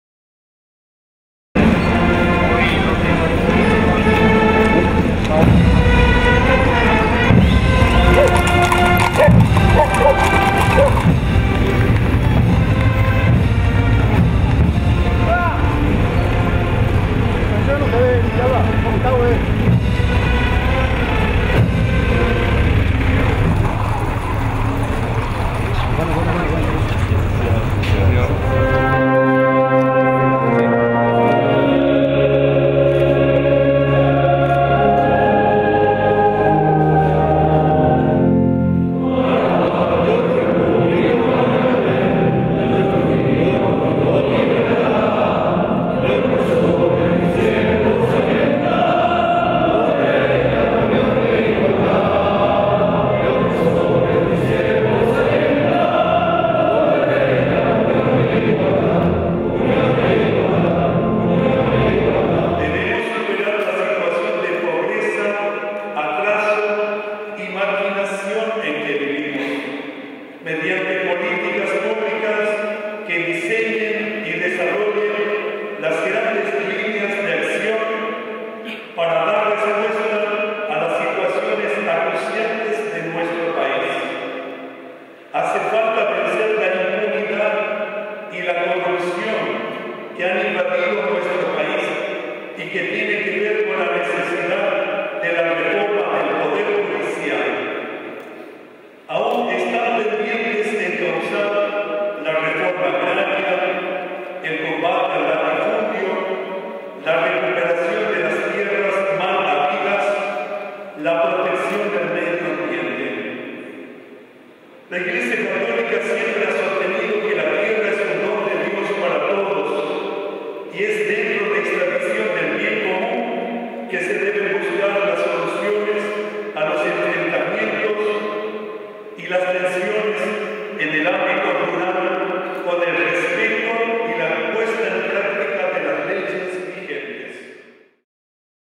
Iglesia insta a vencer la impunidad y corrupción en Paraguay
Asunción, IP Paraguay.- Hace falta vencer la impunidad y la corrupción que ha invadido el país y es necesaria una reforma del Poder  Judicial, sostuvo monseñor Edmundo Valenzuela, durante su homilía en la Catedral Metropolitana por conmemorase los 201 año de Independencia de Paraguay.

Valenzuela manifestó que se debe superar la situación de pobreza, atraso y marginación en que vivimos mediante políticas publicas que enseñen y desarrollen la grandes líneas de acción para dar respuesta a los problemas acuciantes de nuestro país.

"Hemos superado muchas adversidades con la fuerza de la unidad y el aprecio a las personas, a las familias, a las instituciones, a la nación; sin embargo  una actitud egoísta constituye una amenaza para el desarrollo del país", expresó.

Sostuvo que está pendiente la realización de una reforma agraria que incluya el combate al latifundio, la recuperación de las tierras malhabidas y la protección del medio ambiente.

"La iglesia católica siempre sostuvo que la tierra es un don de Dios para todos, y es dentro de esa visión del bien común que se debe buscar soluciones a los enfrentamientos y tensiones en el ámbito rural con el respeto y las justas practicas de las leyes vigentes", refirió el obispo.

Agregó que es urgente afrontar la educación integral de calidad en todo los departamentos del país a igual que la salud publica de calidad al alcances de toda la población paraguaya.

Por ultimo pidió que "sepamos agradecer a Dios por nuestra Patria y sepamos orar por su futuro, sabiendo que la gracia de Dios nunca nos faltará".